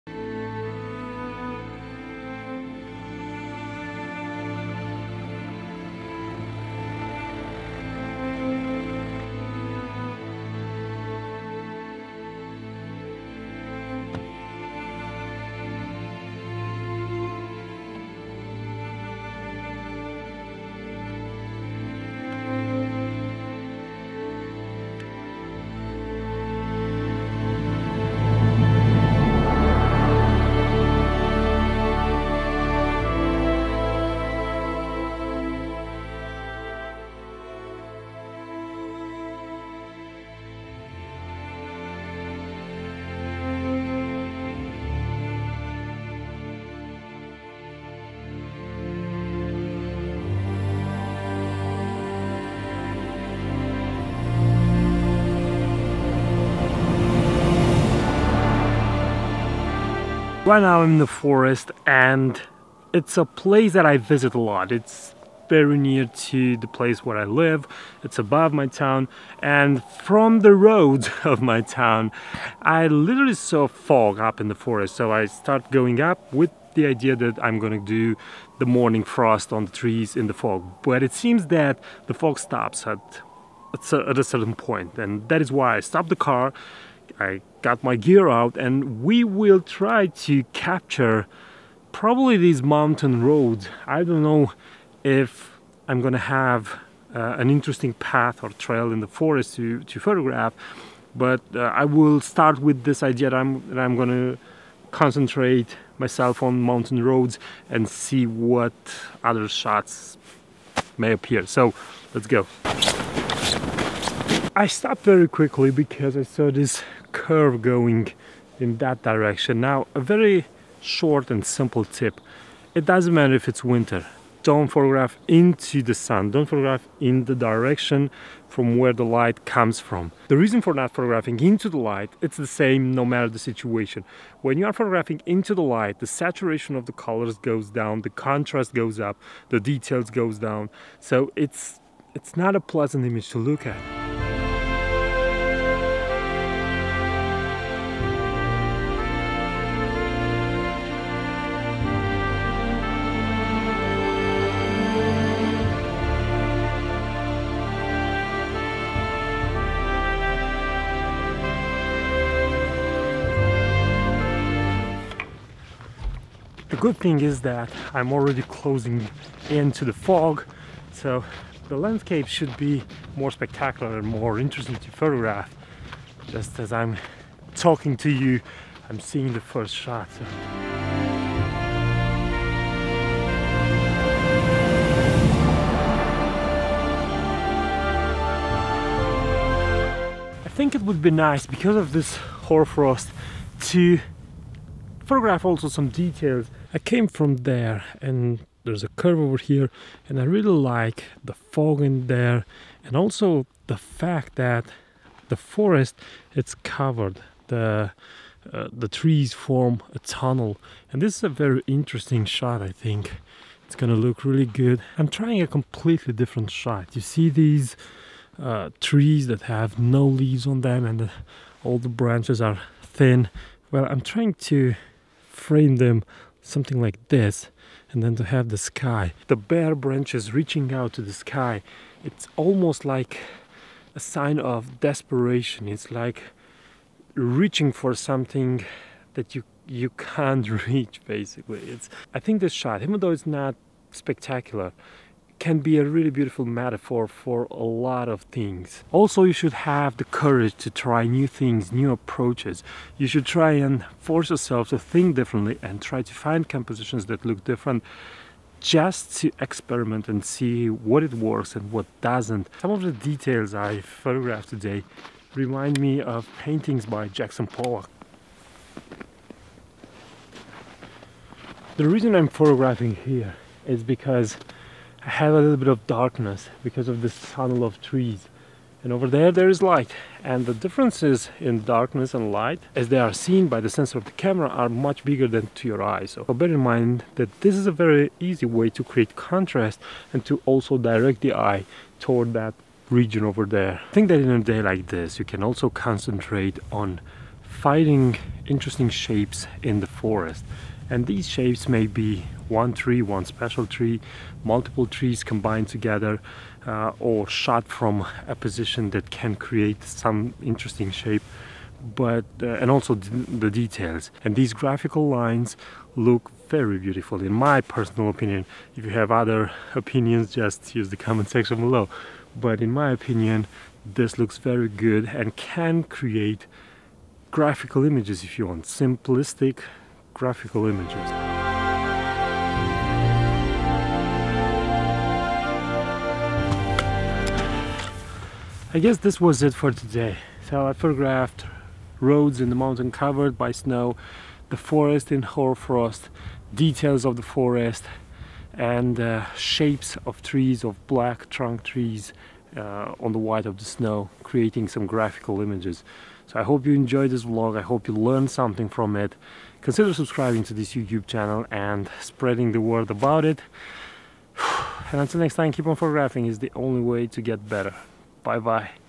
The Hoarfrost Forest - Landscape Photography in the Woods
Today I went into the forest to photograph the frozen trees. The morning hoarfrost and the fog created some interesting opportunities to photograph. Here are some tips on how I approach the situation.